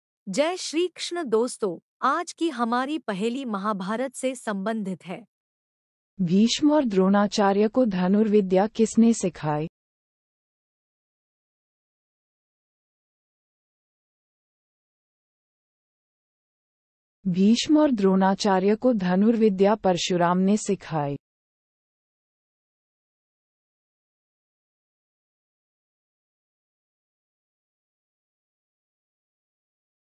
MahabharartQuiz150
Welcome to our educational channel! In this video, we delve deep into the fascinating epic of Mahabharat. We explore the intricate storyline and answer common questions about this ancient Indian mythology. Join us as we unravel the secrets of this legendary tale. Let's embark on this enlightening journey together!